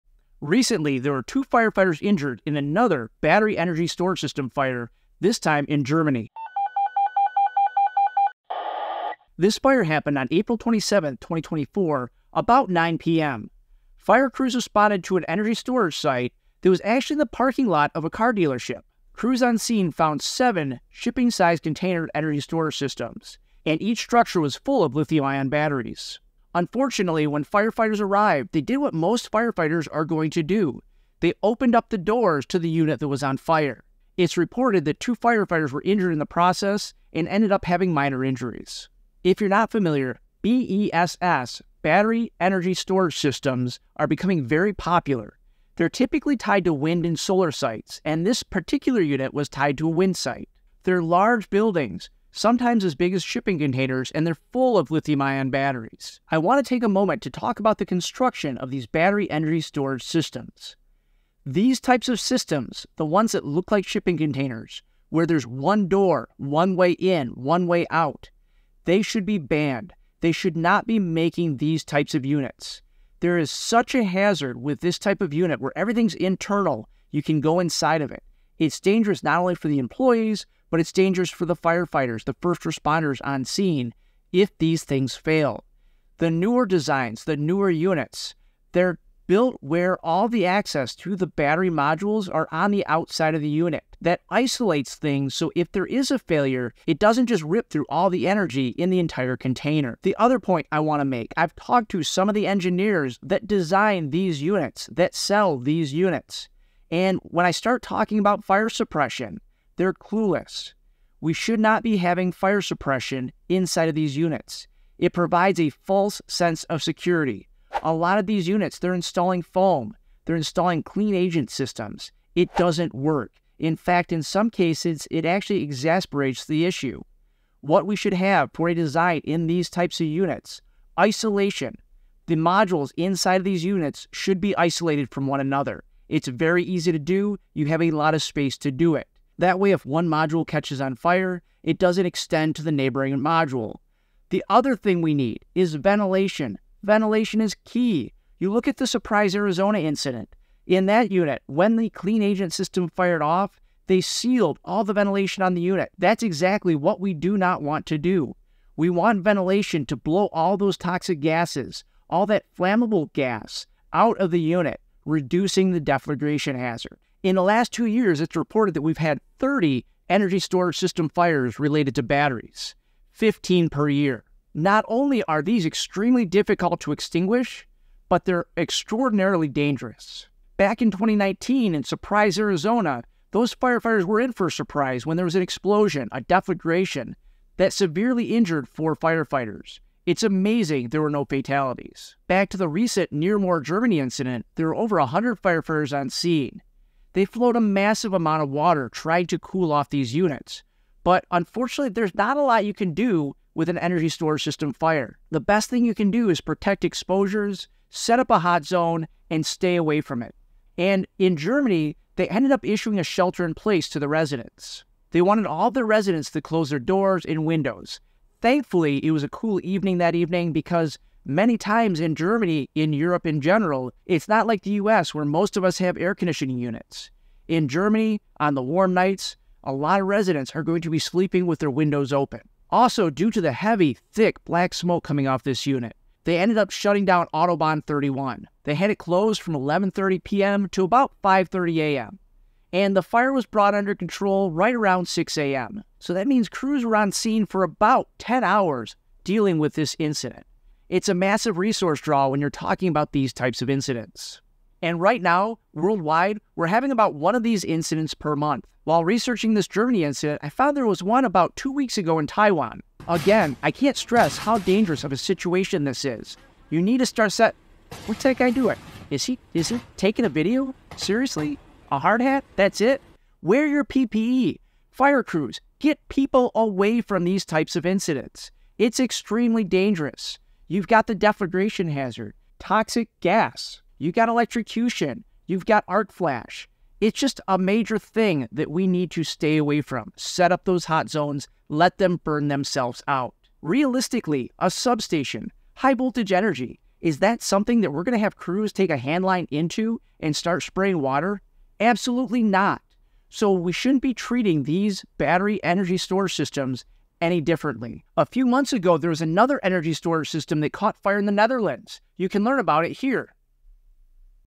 Battery ESS Fire: Neermoor, Germany Incident
Join me as I delve into the recent BESS fire incident in Neermoor, Germany, unraveling the events leading to the thermal runaway and explosions. This comprehensive analysis sheds light on the critical importance of safety protocols in the battery energy storage system industry.

Fire Dept. Coffee Veteran Owned, Firefighter Run

00:00 - Intro
00:38 - What is a BESS (Battery Energy Storage System)
00:55 - BESS Construction
01:39 - BESS Fire Suppression
02:45 - Number of BESS fires per year
03:12 - Neermoor Germany BESS Fire
04:14 - Lanyu Taiwan BESS fire